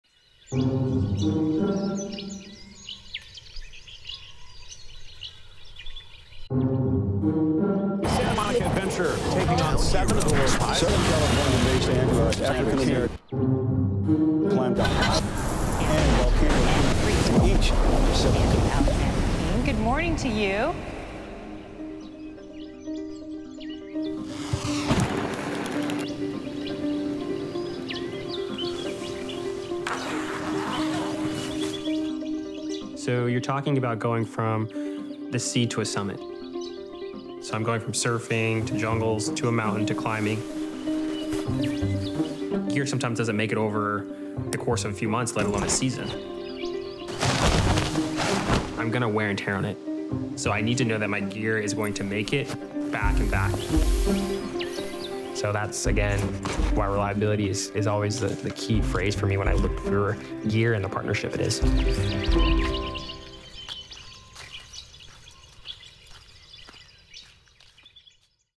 Evac Compression Dry Bag - Adaptability, is the evolution of Adventure
There’s no question: Simple, durable and reliable gear takes you further in the outdoors. With climates that can change in a heartbeat, conditions that can swing without notice, and environments that’ll show you a dozen different personalities, you need to trust in your equipment so you can adapt to your habitat.

That’s where the new EVAC Compression Dry Bag thrives. Primarily made from Bluesign-approved recycled 70D nylon waterproof fabric, it’s the perfect dry storage bag for any adventure, keeping your gear protected from the elements and your mind completely at ease.